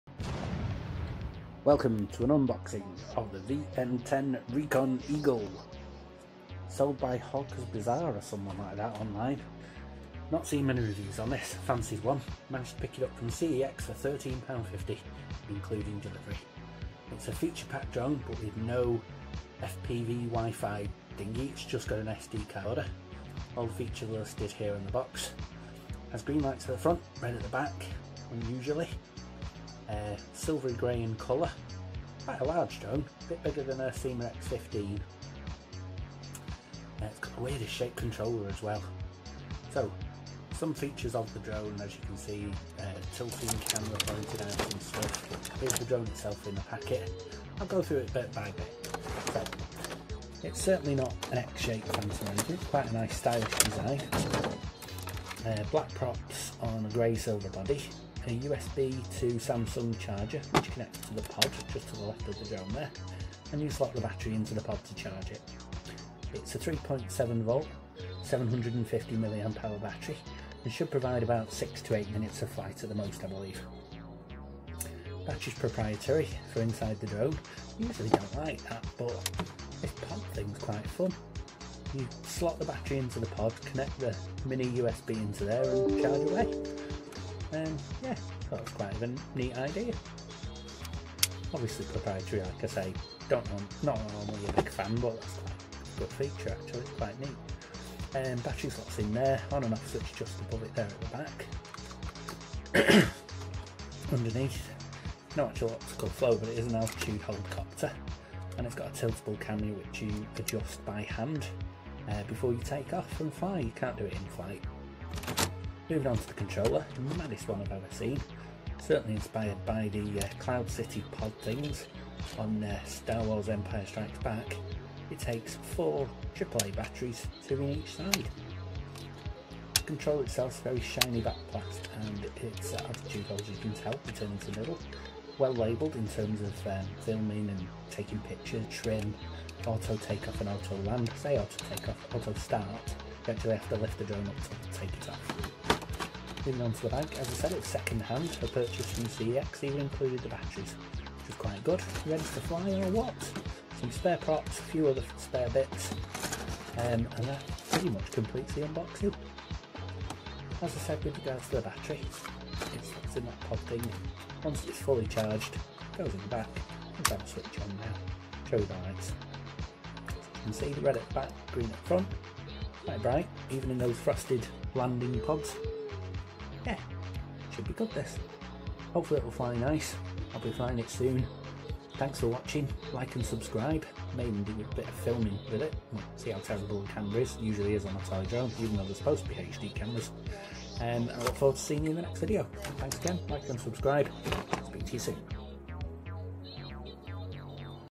VN10 Eagle recon drone unboxing
Camera would not work, being returned sadly, a great and stable flyer but if i ever wanted to sell it on its not worth it.
I also tried to carry a 20 gram camera with it, sadly it struggled so i gave up.